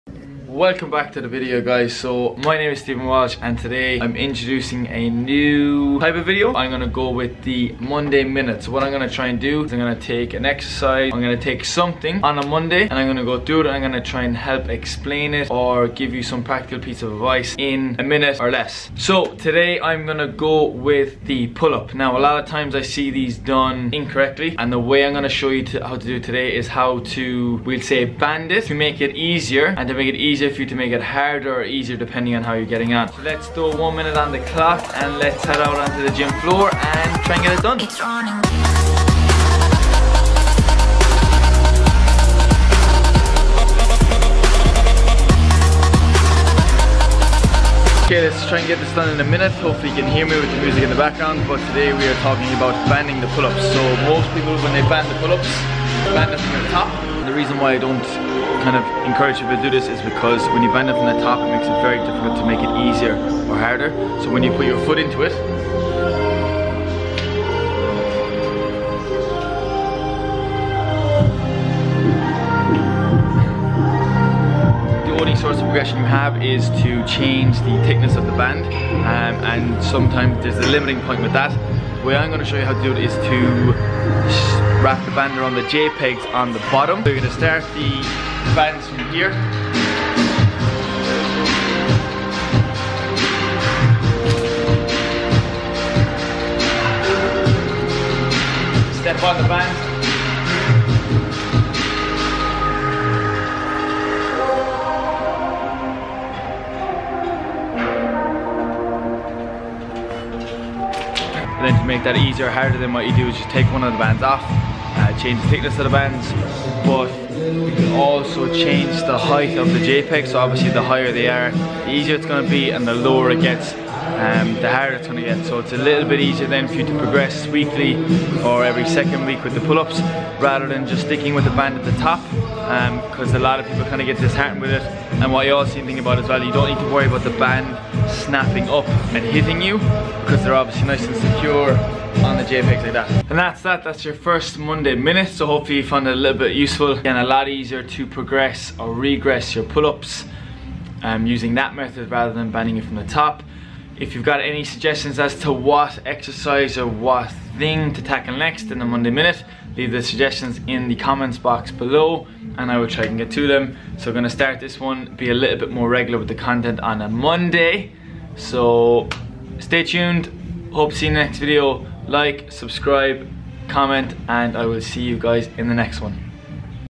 EP 1 ¦ MONDAY MINUTE ¦ START DOING BETTER PULL UPS
New segment on the channel called Monday minute. Going to take an exercise, technique or a topic and try and explain it or give you some sort of practical advice in under 1 minute.

Get your suggestions in in the comments box below. Starting by doing better pull ups!